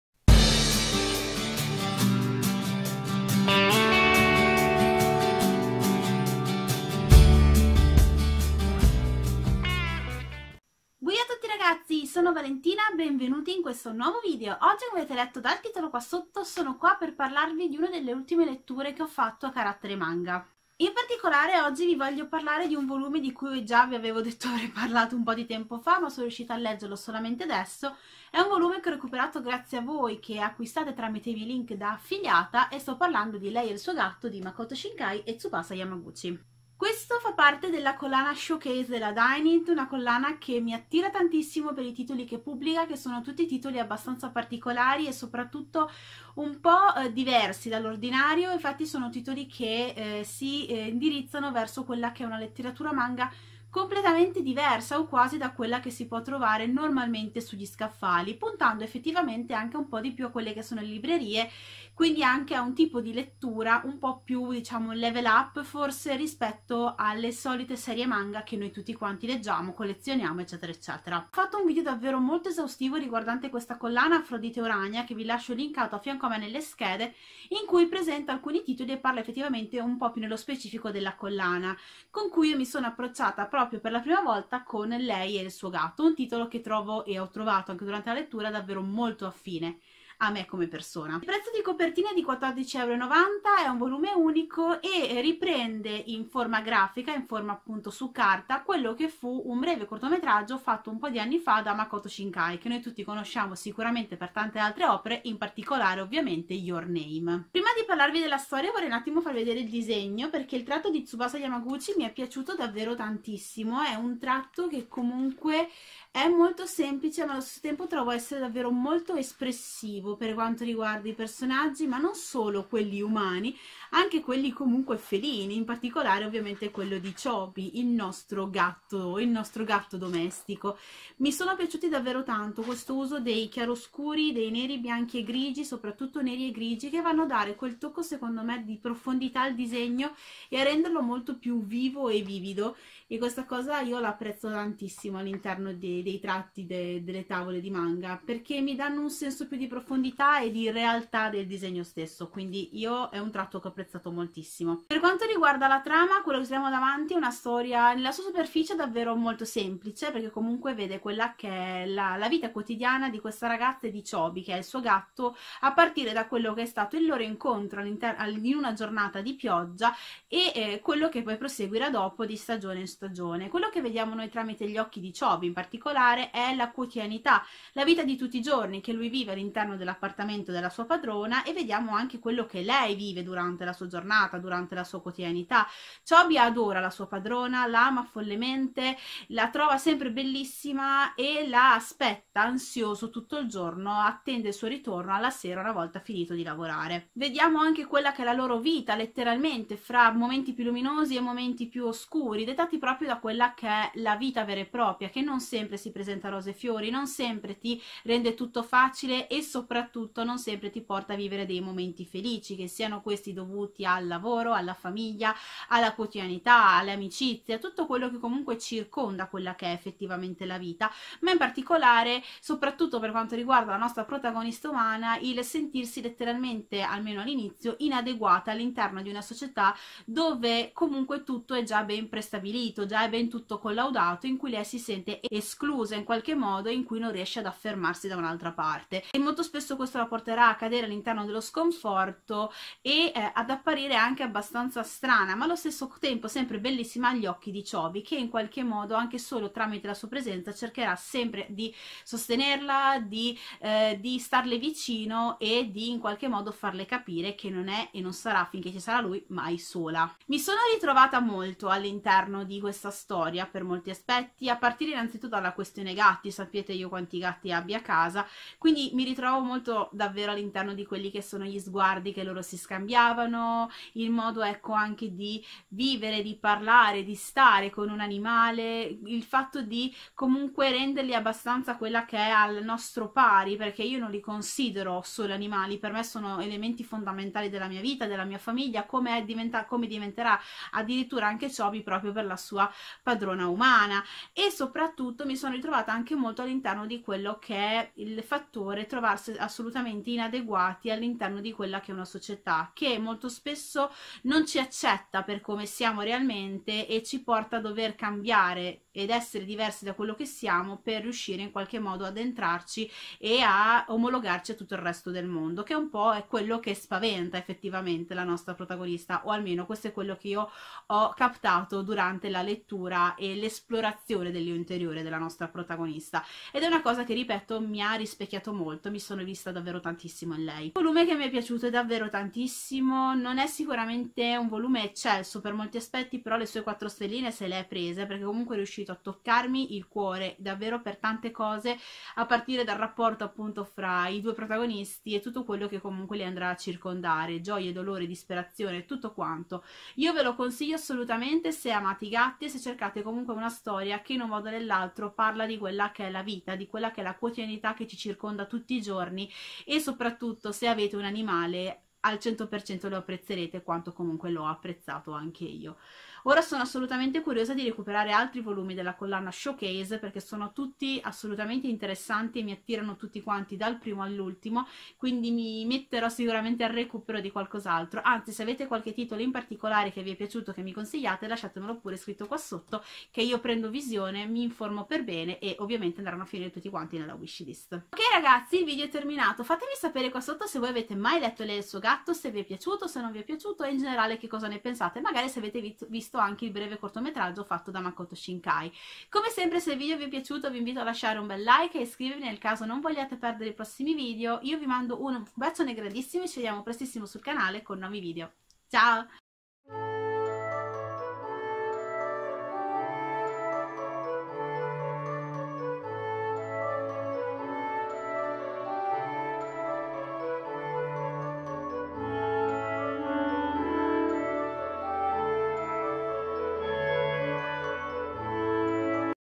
LEI E IL SUO GATTO di MAKOTO SHINKAI 🐈
L'amore che nasce fra un animale ed il suo umano è sempre bellissimo e Makoto Shinkai in questa storia ce lo racconta con una semplicità che tocca il cuore.
L'avete mai letto? Fatemi sapere! Alla prossima :D

LINK AL VOLUME SU AMAZON

--- SOCIAL♥ ---
► Trovate le mie recensioni/articoli sul sito Youbookers.it nella mia rubrica "Letture d'Oriente"